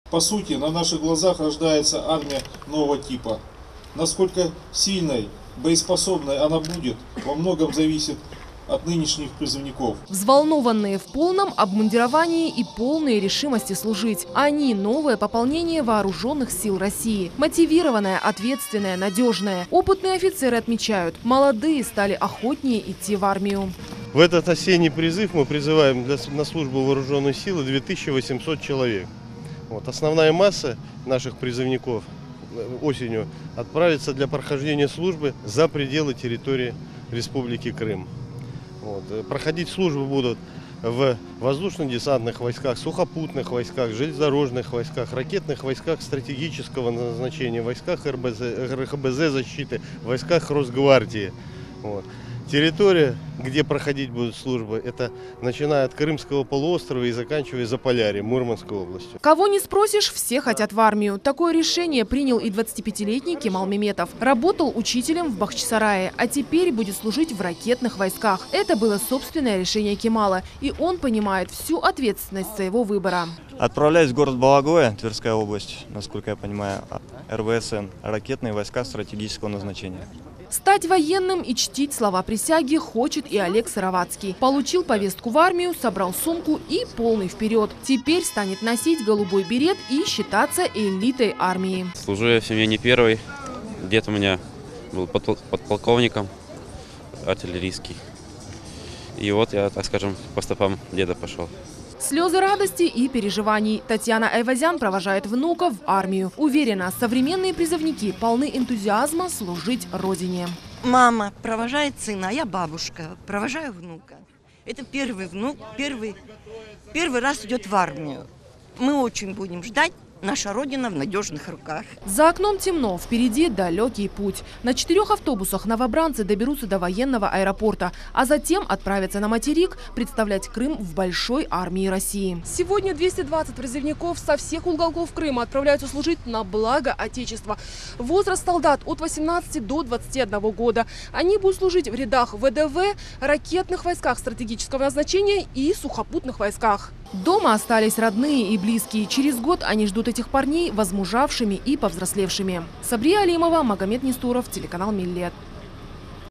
В Крыму подвели итоги осеннего призыва
В Крыму подвели итоги осеннего призыва. Отправка солдат состоялась от сборного пункта военного комиссариата Республики Крым. Срок службы в армии останется неизменным –12 месяцев. А география службы расширилась от Крыма до Заполярья. Как наши ребята укрепят военную мощь страны, знает Сабрие Алимова.